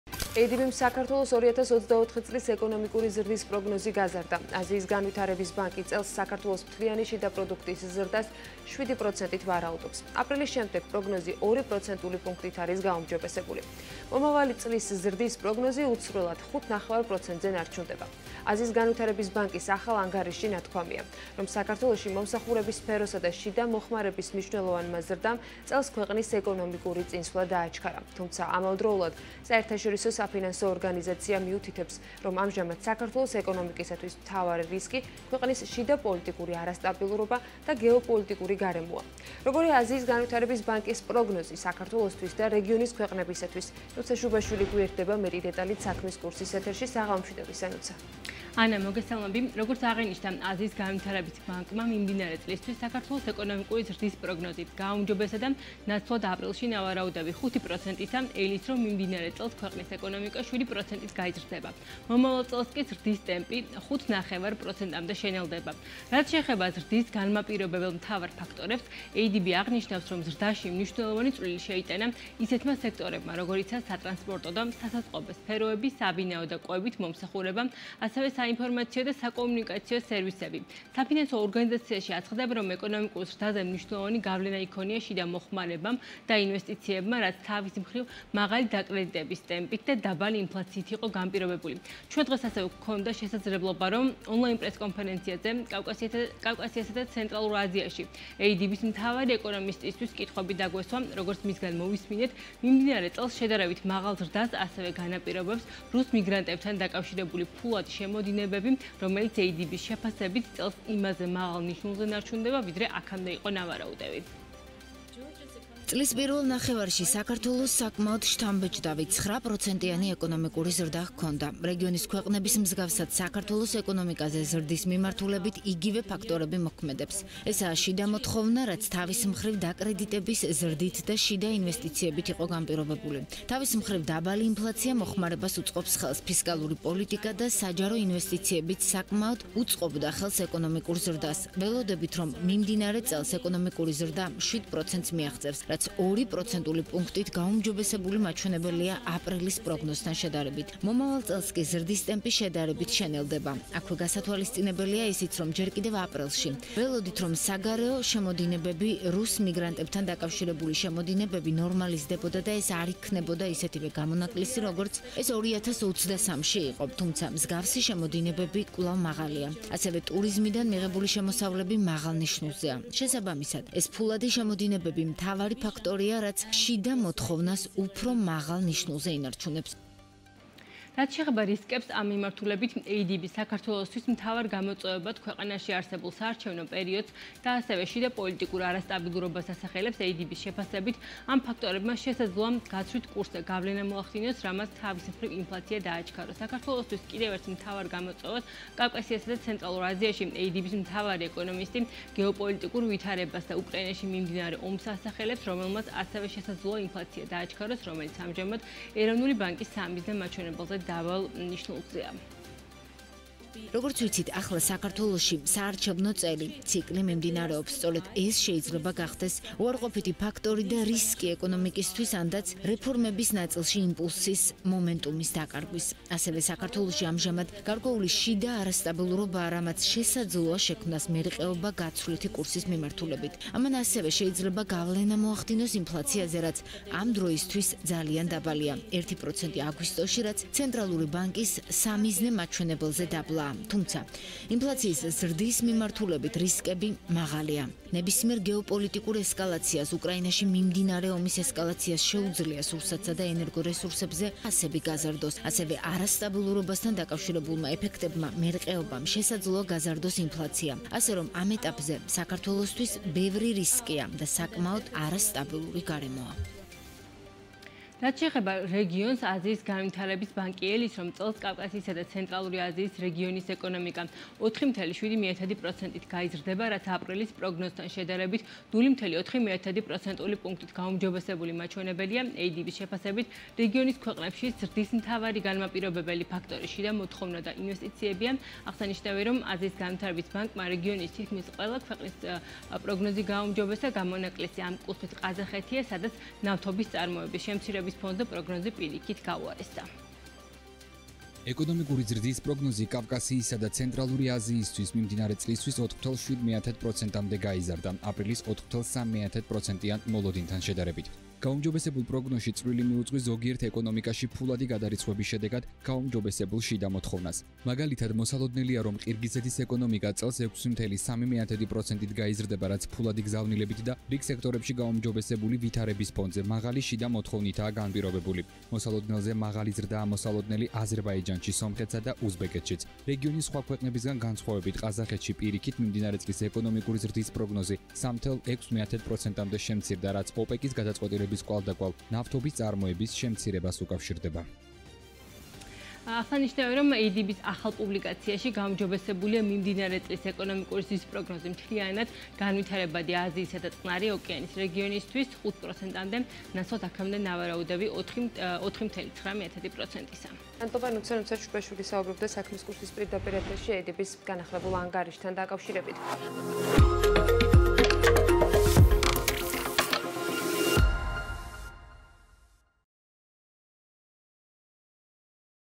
ADB-მ საქართველოს ეკონომიკური ზრდის პროგნოზი 7%-მდე გაზარდა - მიზეზები;
ADB-იმ საქართველოს 2024 წლის ეკონომიკური ზრდის პროგნოზი გაზარდა. აზიის განვითარების ბანკი წელს  საქართველოს მთლიანი შიდა პროდუქტის ზრდას 7%-ით ვარაუდობს, აპრილის შემდეგ პროგნოზი 2 პროცენტული პუნქტით არის გაუმჯობესებულია. 2025 წლის ზრდის პროგნოზი კი უცვლელად, 5.5%-ზე ნარჩუნდება. აზიის განვითარების ბანკის ახალ ანგარიშში ნათქვამია, რომ საქართველოში მომსახურების სფეროსა და შიდა მოხმარების მნიშვნელოვანმა ზრდამ 2024 წელს ქვეყნის ეკონომიკური წინსვლა დააჩქარა. თუმცა ამავდროულად, საერთაშორისო საფინანსო ორგანიზაცია აღნიშნავს, რომ ამჟამად საქართველოს ეკონომიკისთვის მთავარი რისკი ქვეყნის შიდაპოლიტიკური არასტაბილურობა და გეოპოლიტიკური გარემოა. როგორია აზიის განვითარების ბანკის პროგნოზები საქართველოსთვის და რეგიონის ქვეყნებისთვის.